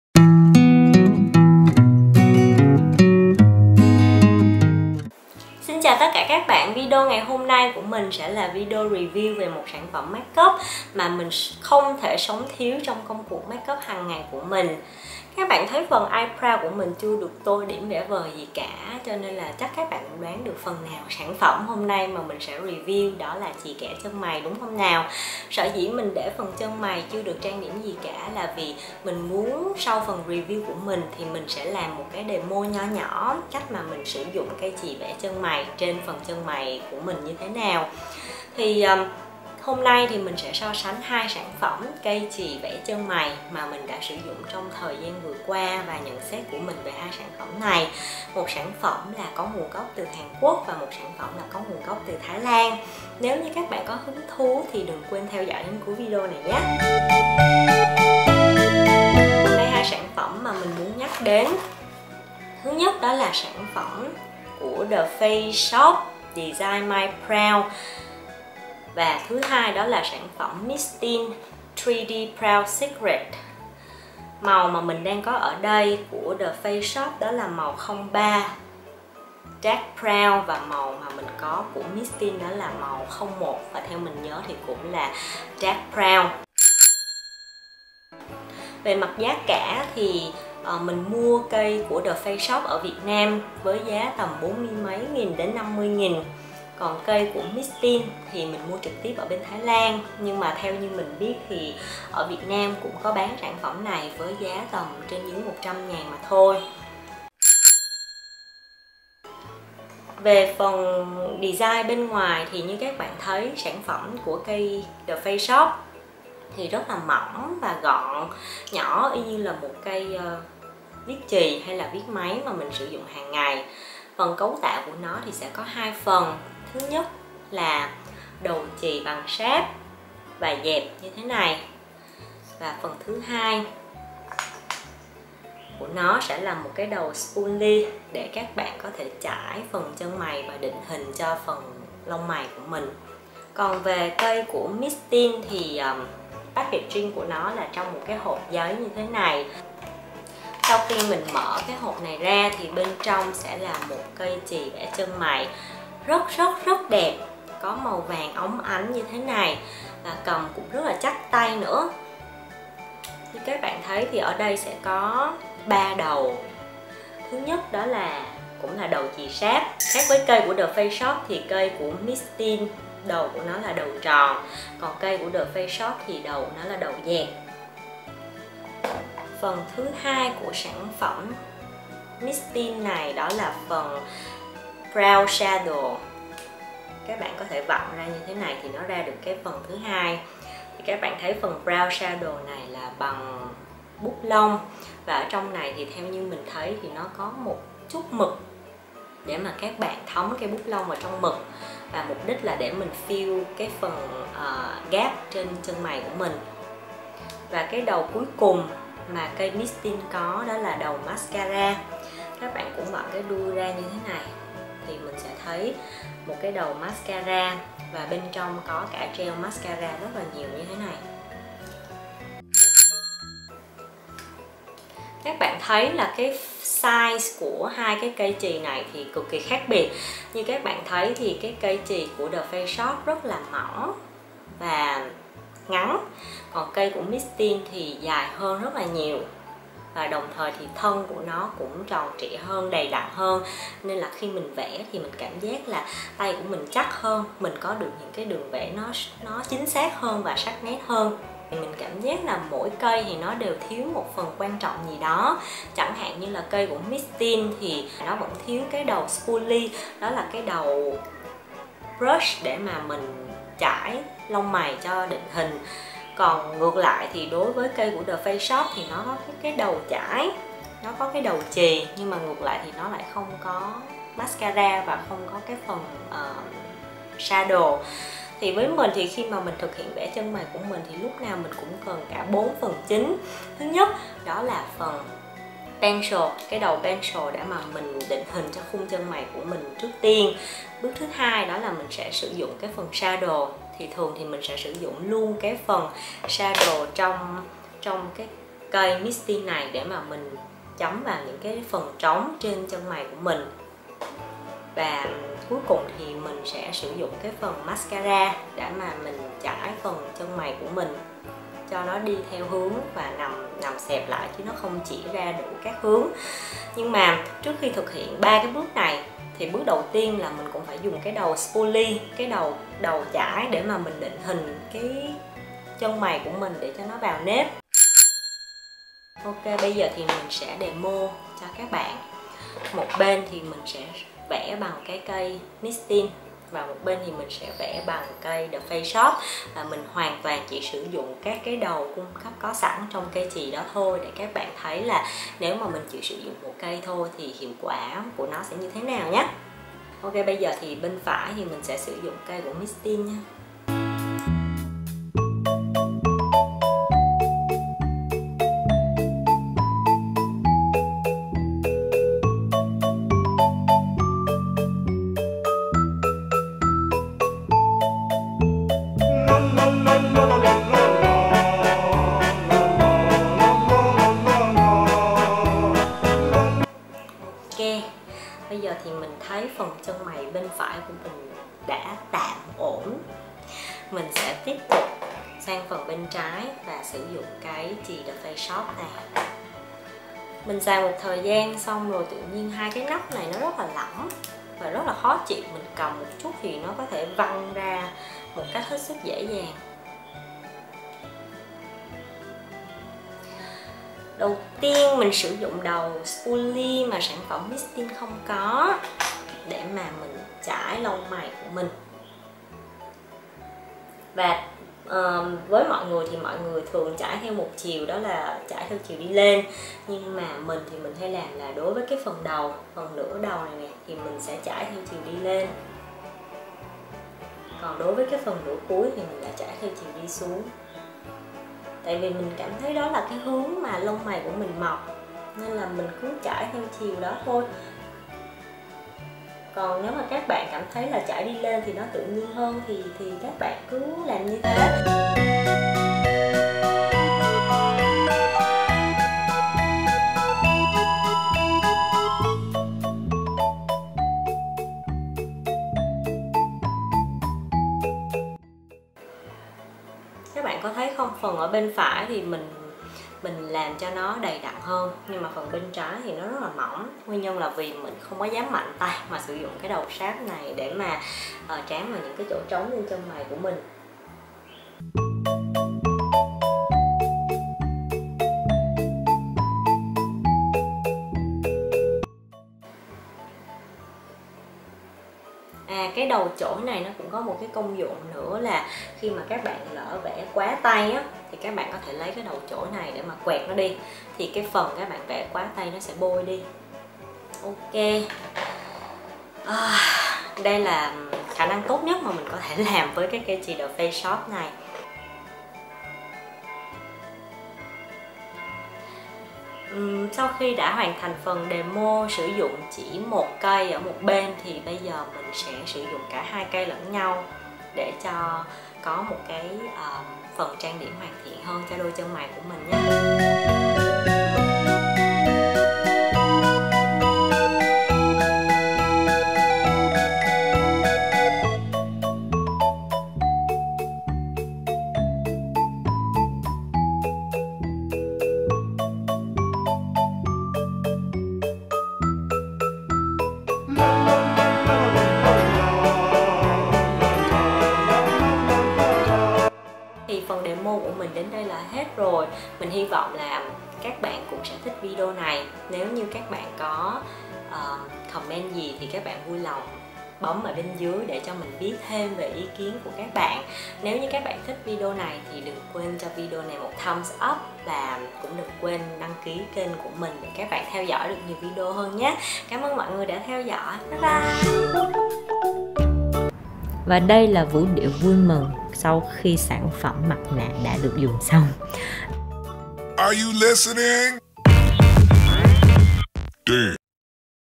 Review Brow Pencil Products | Review Các Sp chì Kẻ Chân Mày TheFaceShop & Mistine | Petitemeow257
The battle between...
❤  The Faceshop Design My Brow
❤ Mistine 3D Brow Secret